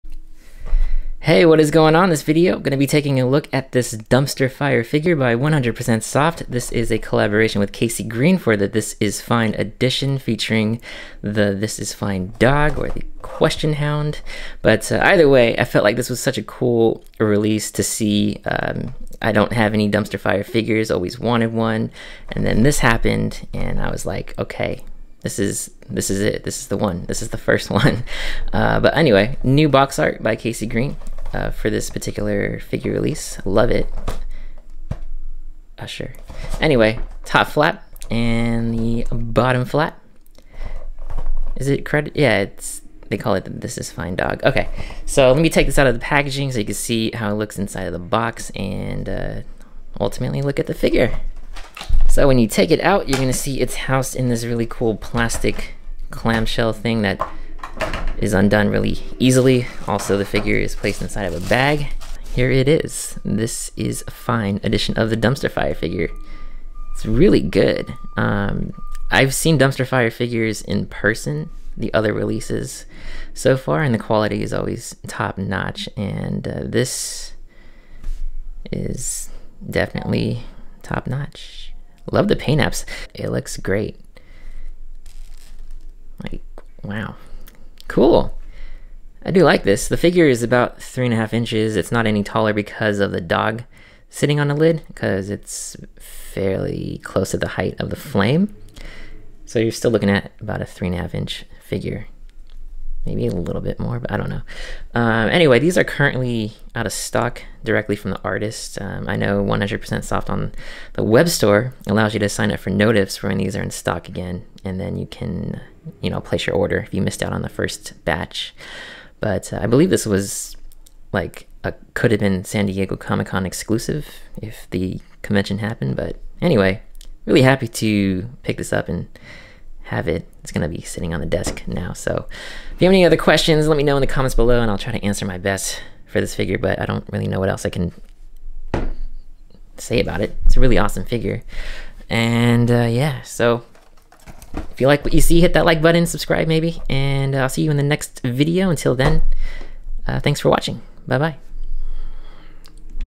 Dumpster Fire 'This is Fine' Vinyl Figure by 100% Soft & KC Green Review
Really love this new Dumpster Fire vinyl figure. The collab between 100% Soft and KC Green is straight up fire..